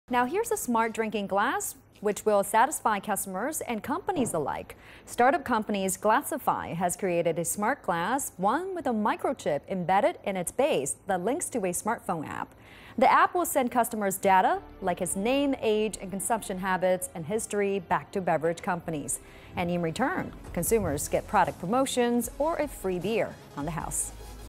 Smart beer glass taps into drinkers' data
Startup company Glassify has created a smart glass, with a microchip embedded in its base that links to a smartphone app. The app will send customers' data, like name, age, consumption habits and history, back to beverage companies. And in return, consumers get product promotions, or a free beer on the house.